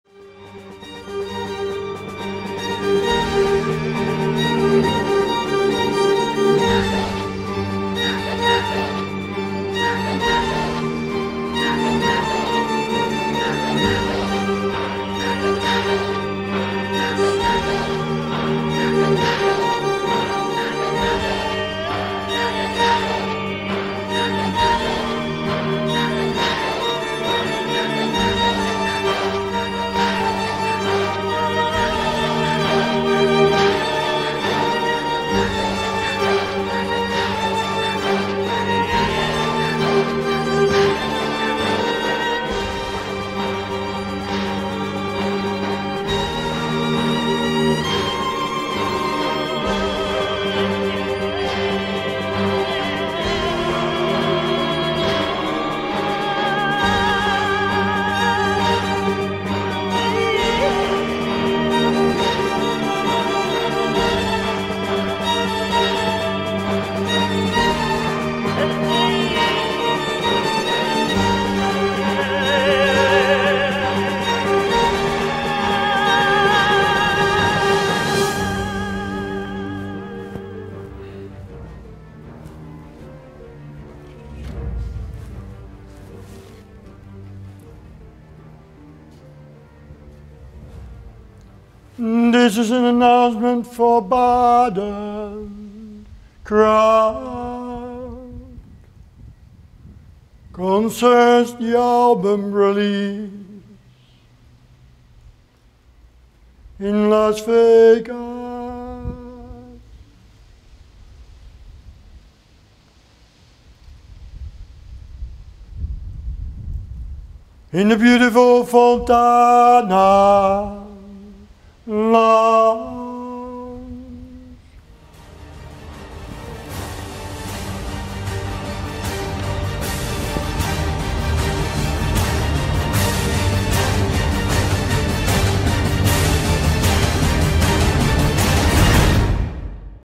Marcel Luske: Baden Announcement
Marcel Luske was in Baden, Austria as team captain for the Dutch National Poker Team, to compete in the Nations Cup. But that's not all he did there. He also had a very special announcement for his fans.